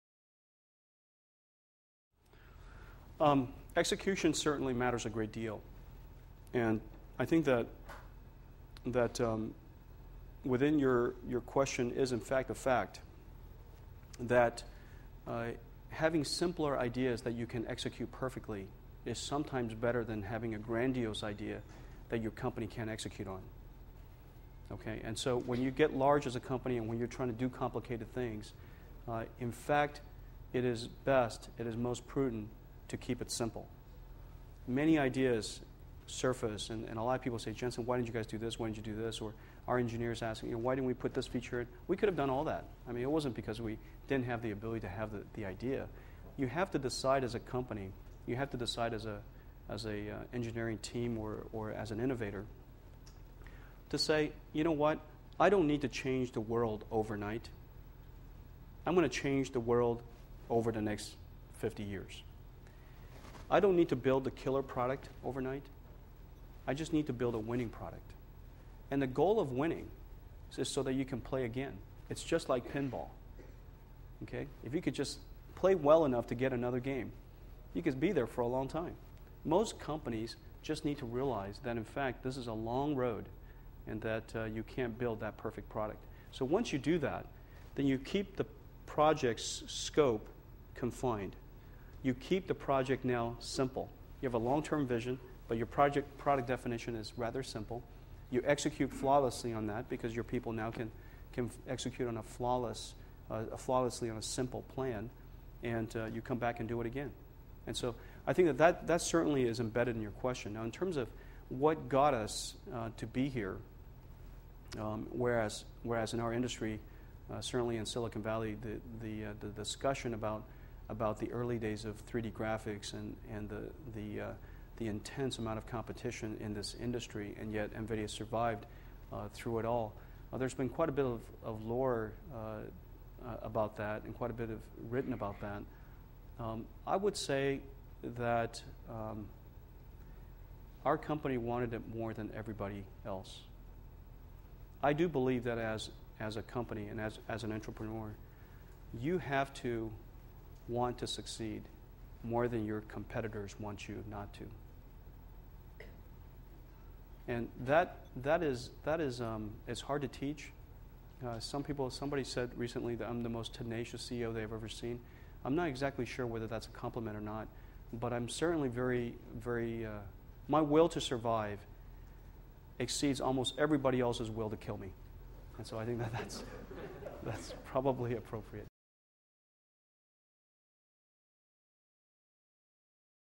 Jensen Huang-The Importance of Execution
Execution is critically important; it is better to have a simple idea that can be easily implemented rather than a complicated idea that has implementation challenges.  Drastic changes do not have to be made overnight; it is a long road to success a